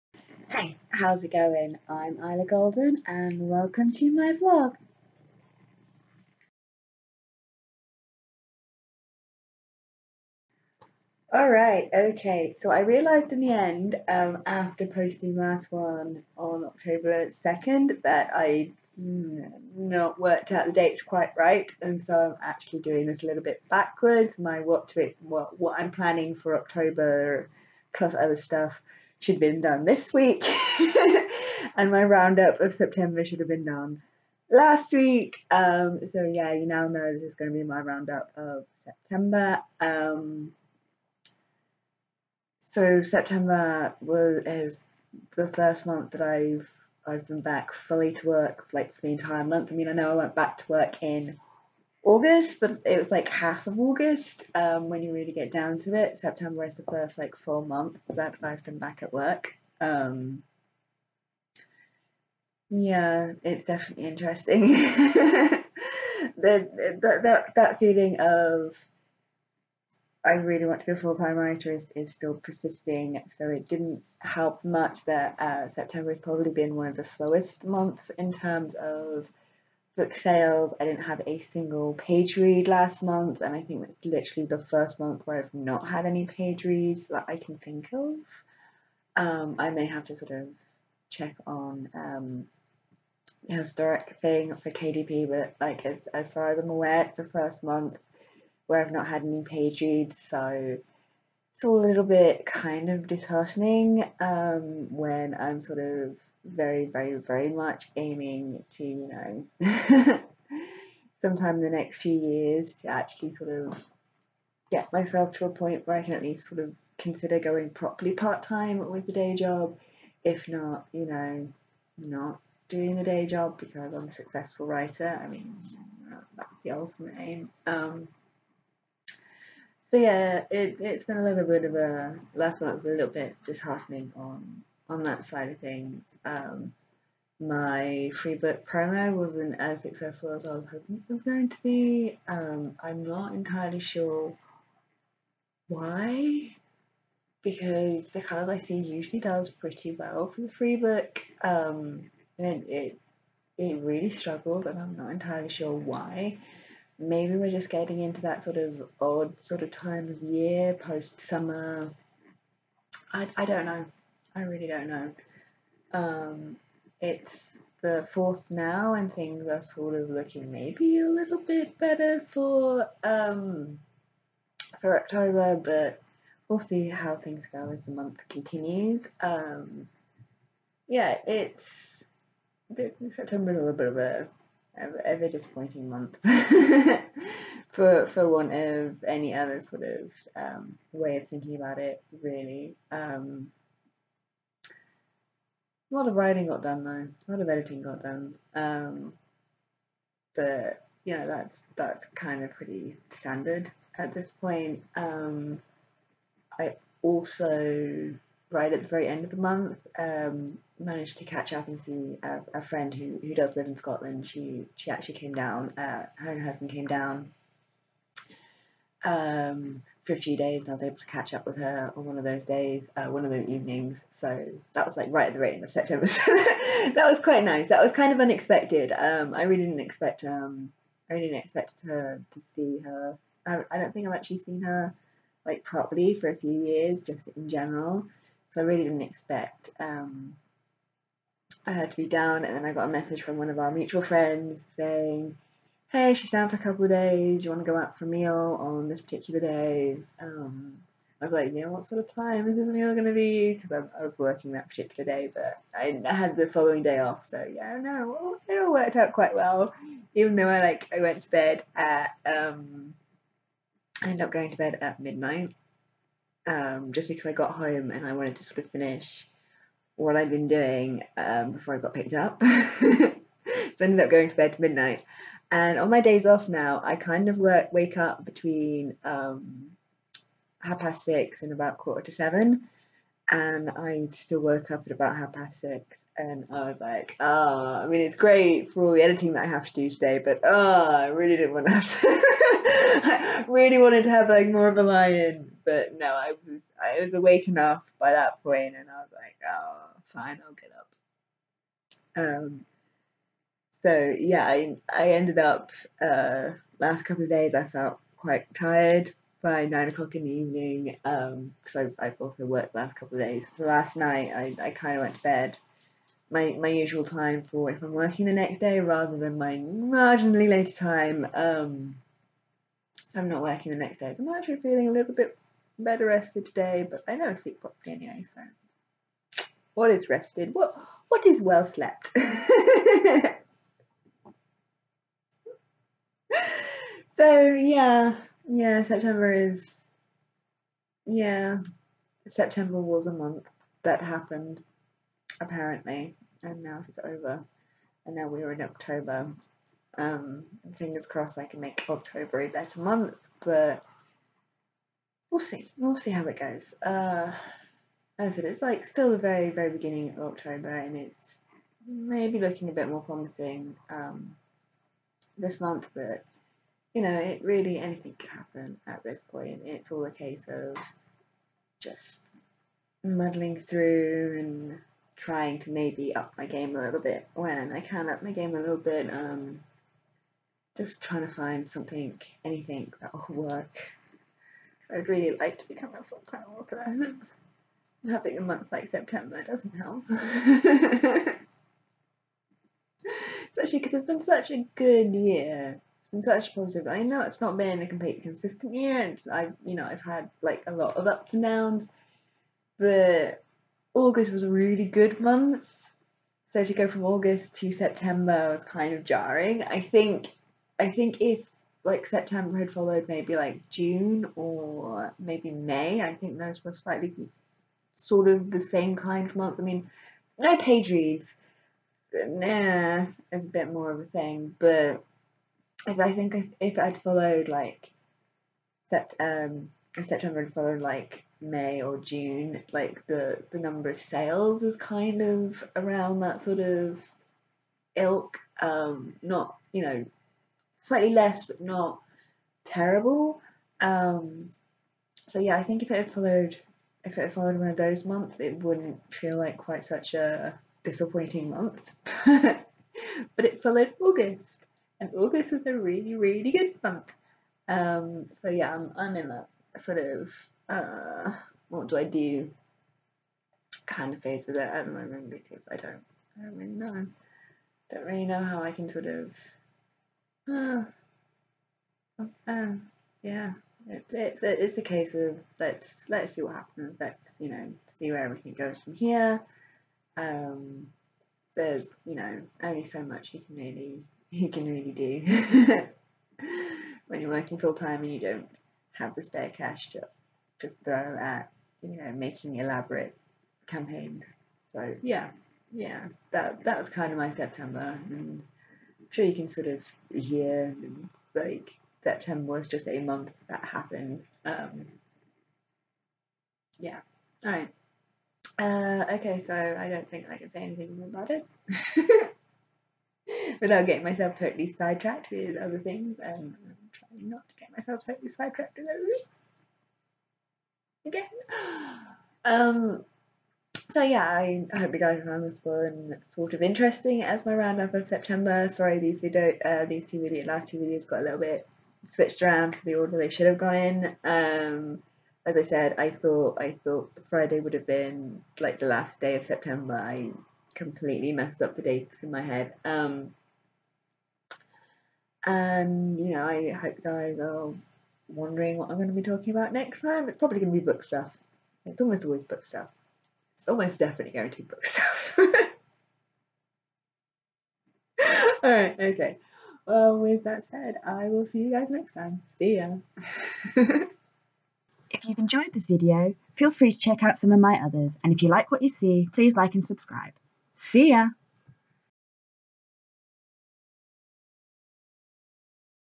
Vlog 197 So September Happened...
My roundup of September... yes I should have done this before last weeks, woops XD

Want a copy of one or more of my books, here's a website that'll get you where you need

If you're interested in checking out the fanfic saga I mentioned, this is the link to the first book in the saga, now on ff.net